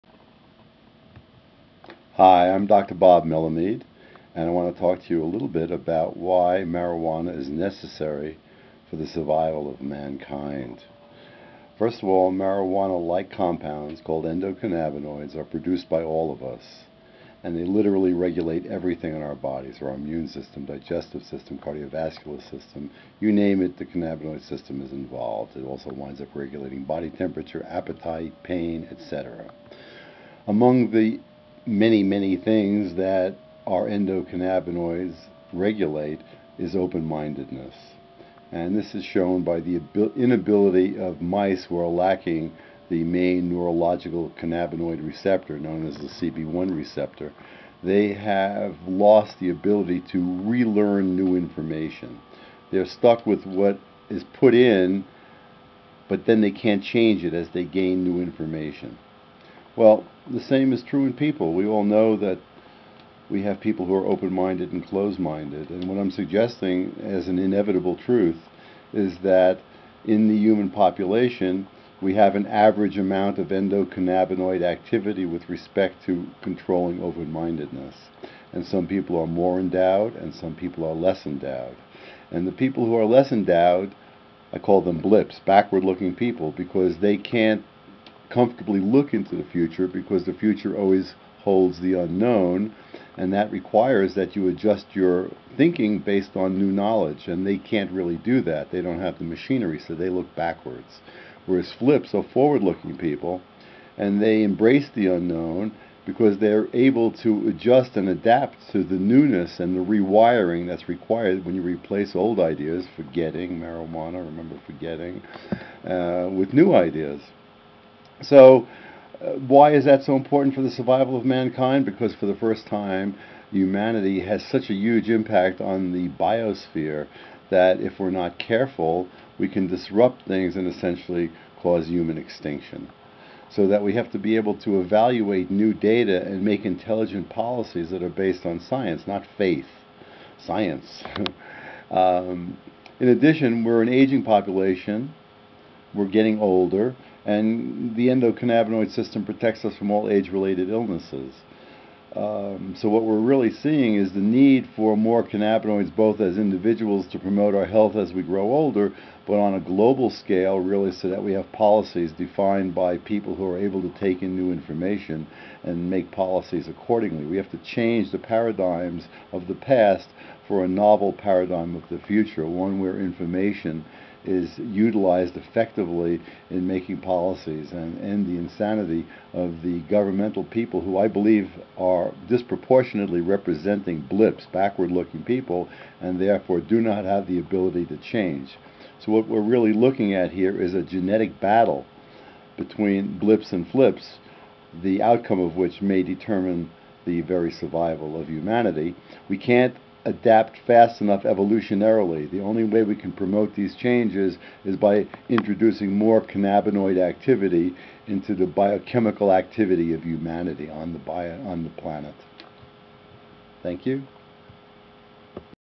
BLPs and FLPs, The Biology of Democracy
We are engaged in a genetic battle between backward looking people (BLPs) and forward looking people (FLPs). Your cannabinoid activity determines your level of open-mindedness.